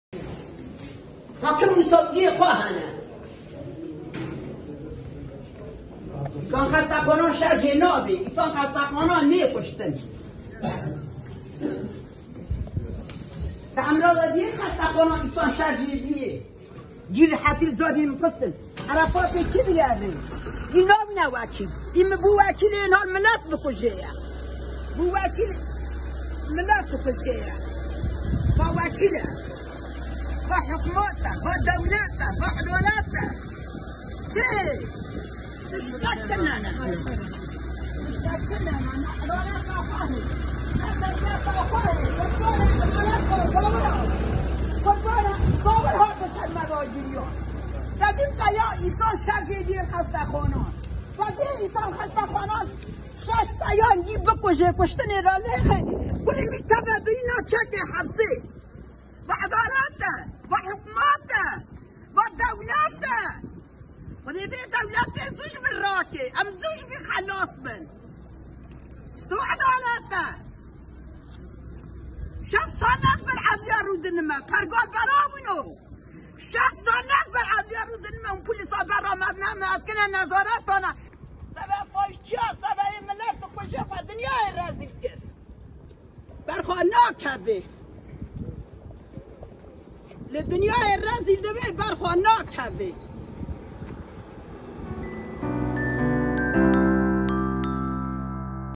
FADIL ŞENYAŞAR TAHLİYE EDİLMEDİ: ANNE EMİNE ŞENYAŞARIN TEPKİSİ
Riha'nın Pirsûs (Suruç) ilçesinde 14 Haziran 2018 tarihinde AKP eski Milletvekili İbrahim Halil Yıldız’ın koruma ve yakınları tarafından Şenyaşar ailesine yönelik gerçekleştirilen saldırıya dair davanın duruşması görüldü. Anne Emine Şenyaşar'ın tahliye edilmesi için Adalet Nöbeti tuttuğu Fadıl Şenyaşar, bu duruşmada da tahliye edilmedi.